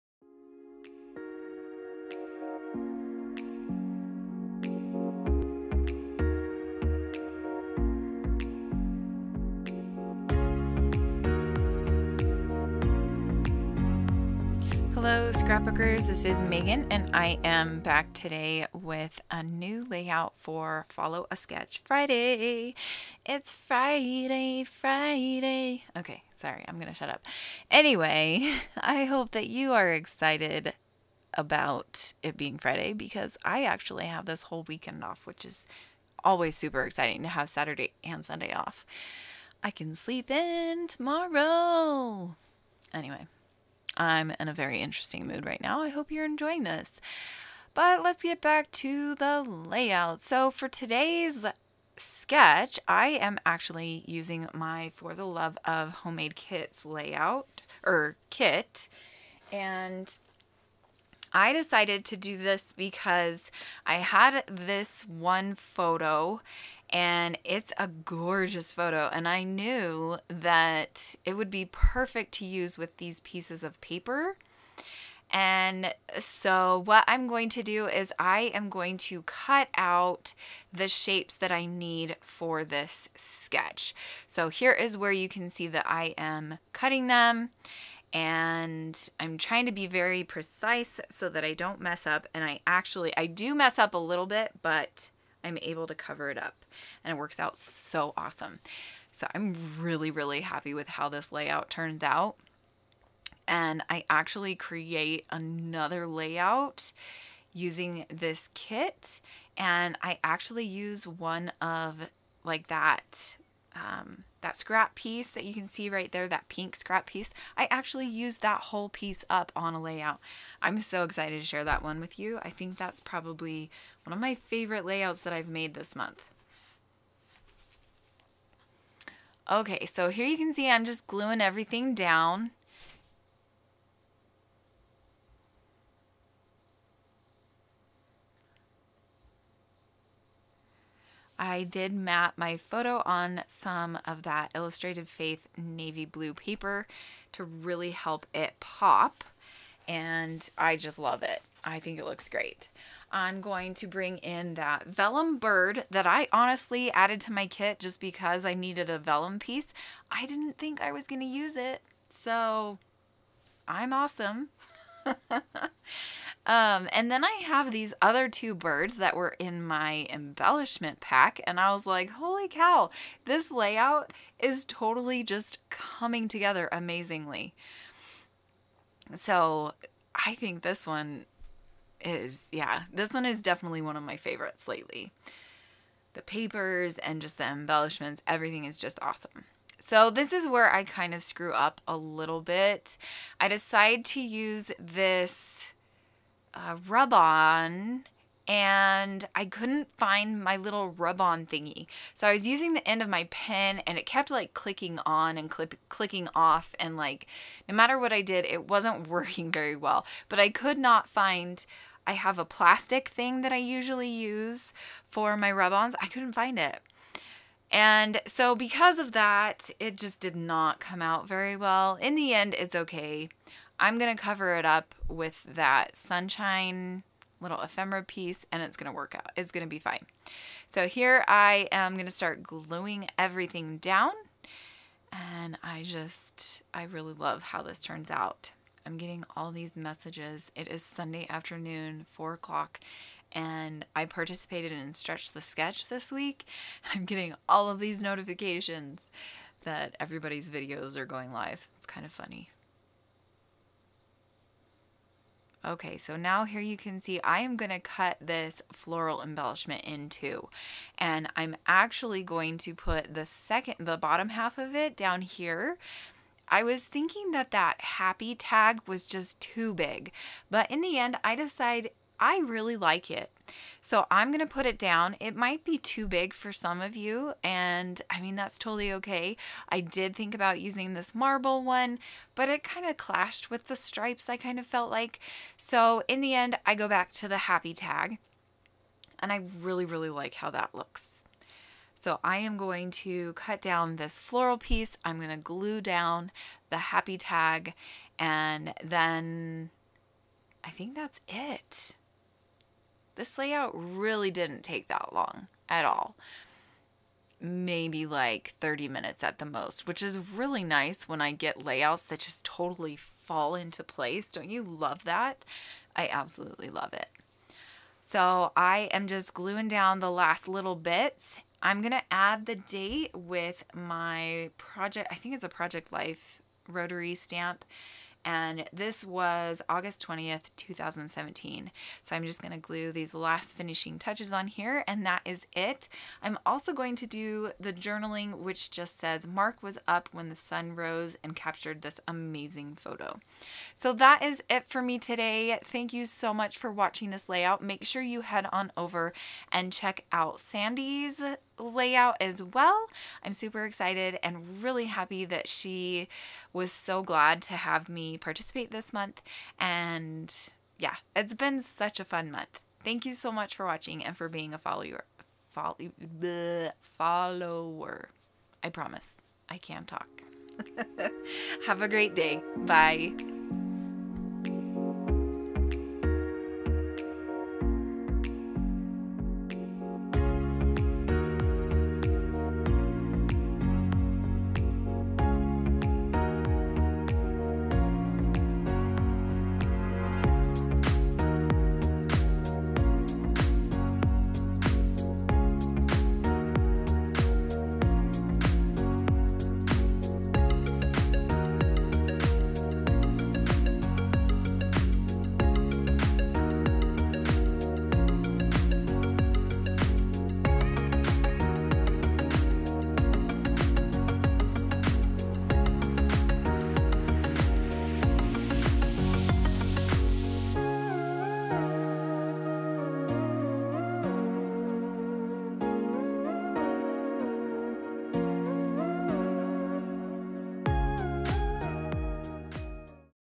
FOLLOW A SKETCH FRIDAY | 12X12 Layout Process Video | Sunshine Love
Hello scrapbookers!! I'm so excited to share this layout with you today!! This one turned out so amazing, even more than I thought it would. Dont you love it when that happens with a layout?!